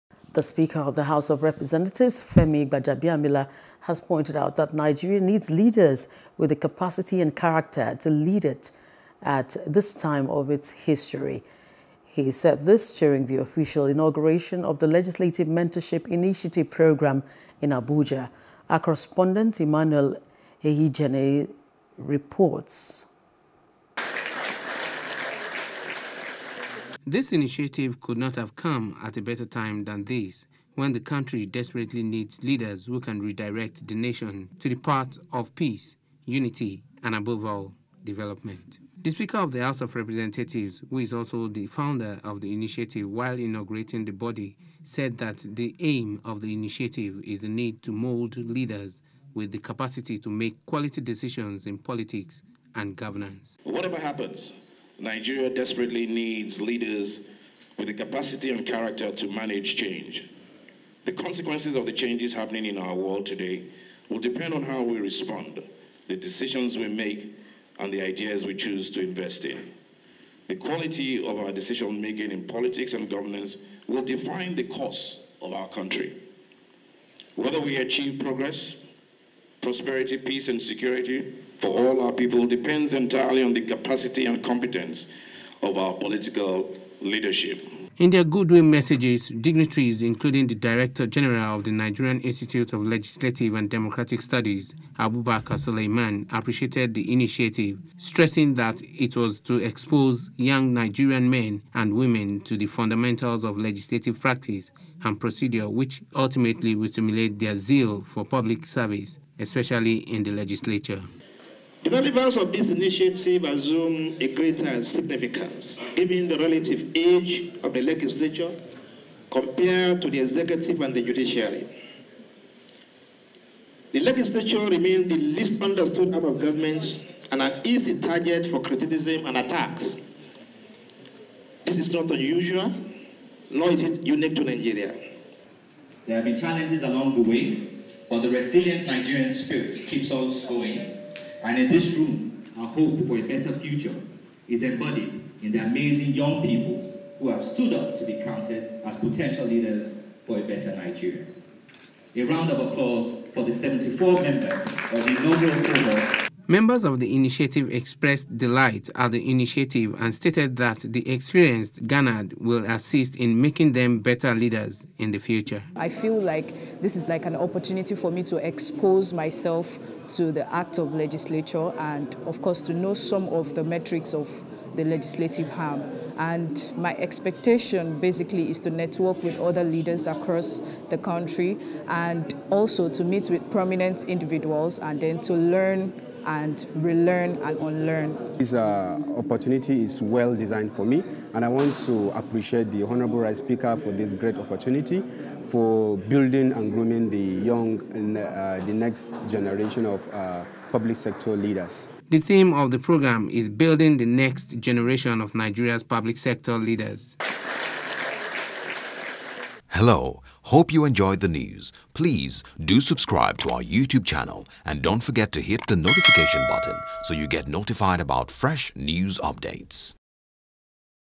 Legislative Mentorship: Nigeria Needs Leaders With Capacity And Character - Gbajabiamila | NEWS
The speaker of the house of representatives, Femi Gbajabiamila has pointed out that Nigeria needs leaders with the capacity and character to lead it at this time of its history.